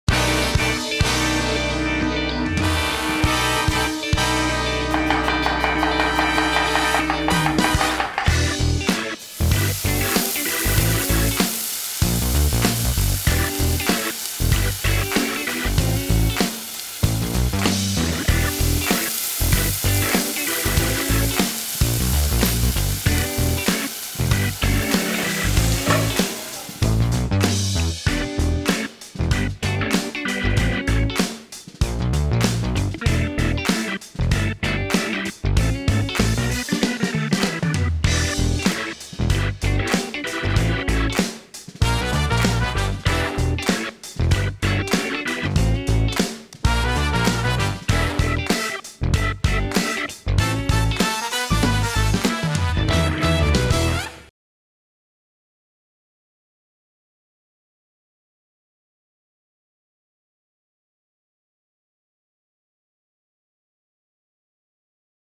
Vegan Double BBQ Cheeseburger | Wicked Healthy
The vegan burger of your dreams, doubled! Field Roast Fieldburgers topped with onions, mushrooms, Chao cheese and BBQ sauce. Unhinge your jaw and savor the flavor!